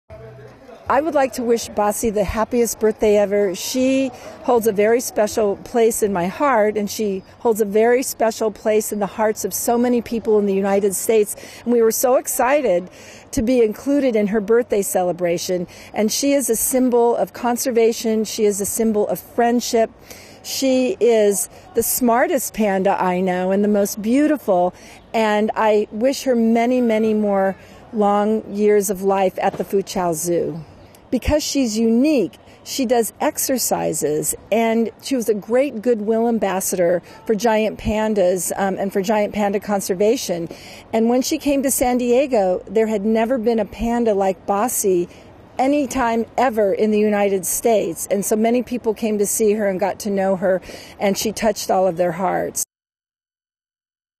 Birthday wishes for Basi, oldest known giant panda
The oldest known giant panda in the Chinese mainland celebrates her 35th birthday on Saturday -- the equivalent of 130 years in human age.

Basi may be the most famous panda in China.

She first rose to international fame as the mascot of the 11th Asian Games in Beijing in 1990.

She was trained to ride a bicycle, shoot basketball hoops, and lift weights. In 1987, she stayed in San Diego, USA for half a year, where her stunts fascinated many visitors.

She now resides at a research and exchange center in Fuzhou, capital of southeast China's Fujian Province.